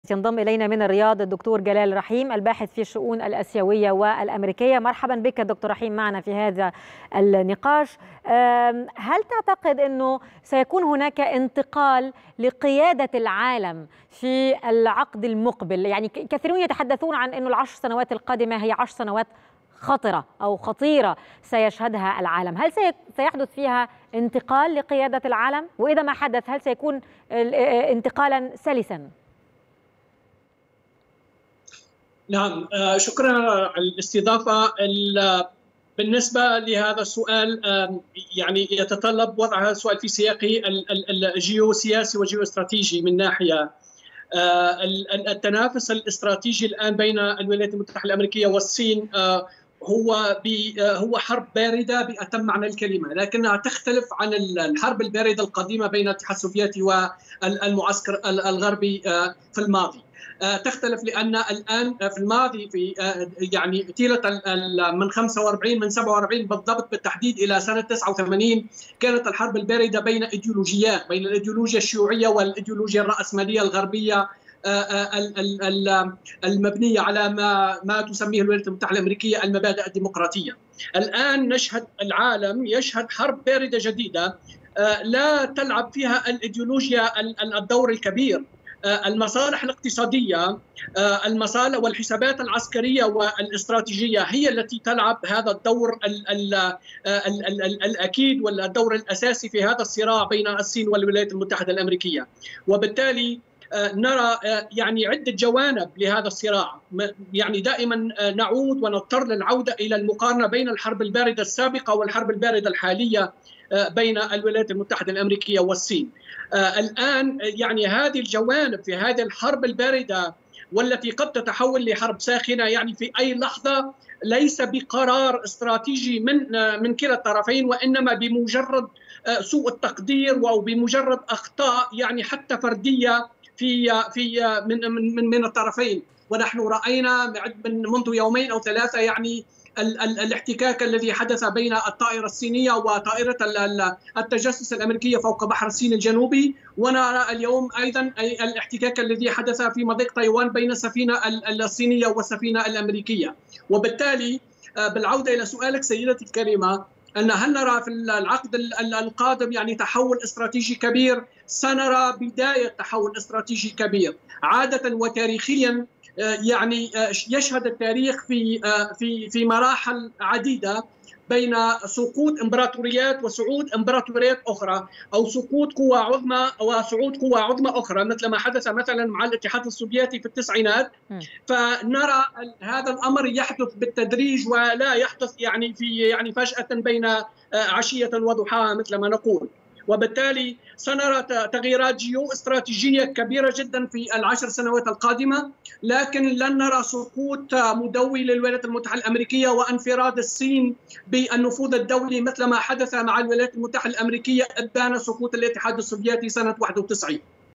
د. جلال رحيم باحث في الشؤون الآسيوية: الصراع بين الصين وأمريكا قد يتحول في لحظة إلى حرب شاملة
4/6/2023